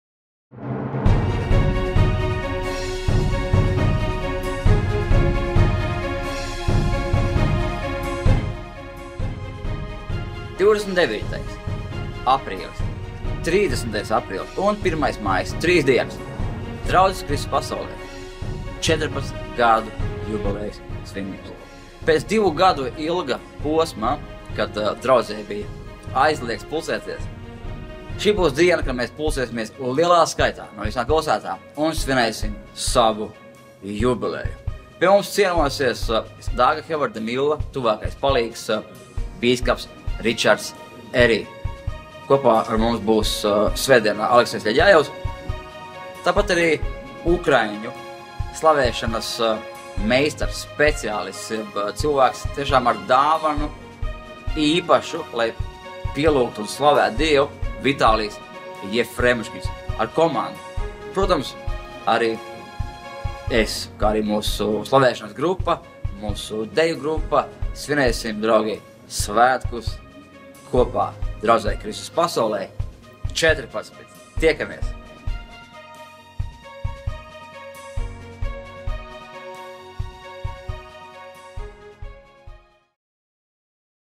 14 gadu jubileja Kristus Pasaulei. 29.04. - 01.05.2022.
Draudzē "Kristus Pasaulei" no 29. aprīļa līdz 1. maijam notiks draudzes 14 gadu jubilejas konference.

Konferences programma:

Piektdien:
29.04. pl.19:00 dievkalpojums ar mācītāju Mariusu Balciunas

Sestdien:
30.04. pl.14:00 dievkalpojums ar mācītāju Alekseju Ļedjajevu
30.04. pl.18:00 slavēšanas un pielūgsmes vakars ar Vitāliju Jefremočkinu

Svētdien:
01.05. pl.11:00 svētku dievkalpojums ar mācītāju Ričardu Erī  un mācītāju Mārci Jencīti. Slavēšanu un pielūgsmi vadīs draudzes „Kristus Pasaulei” slavēšanas komanda un V. Jefremočkins ar komandu.

Konferencē kalpos:
•Ričards Erī - draudžu apvienības “The Lighthouse Chapel International” bīskaps Londonā un apvienības dibinātāja Dāga Hevarda-Milla tuvākais palīgs.
•Aleksejs Ļedjajevs - draudžu apvienības "Jaunā Paaudze" dibinātājs un mācītājs, dzejnieks, komponists un sabiedriskais darbinieks.
•Vitālijs Jefremočkins - slavēšanas līderis, profesionāls izpildītājs, dziesmu autors, mūziķis un aranžētājs.
•Mārcis Jencītis - draudzes „Kristus Pasaulei” dibinātājs un vecākais mācītājs.

Adrese: Braslas iela 29a, 4. stāvs, D ieeja, Rīga